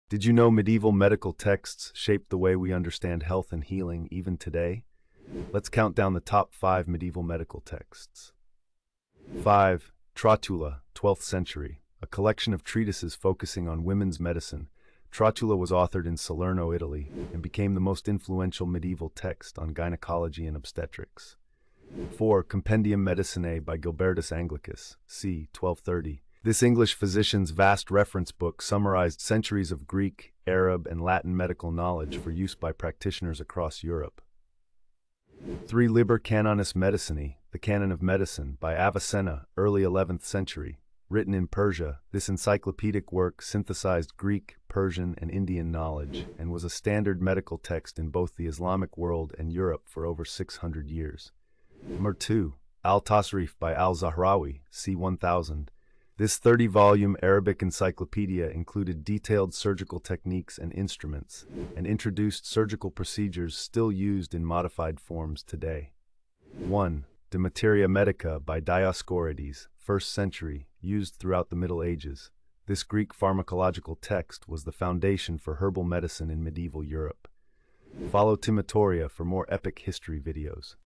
Top 5 Groundbreaking Medieval Medical Texts
Discover the 5 medieval medical texts that revolutionized healing and knowledge in the Middle Ages! Which one do you find most fascinating?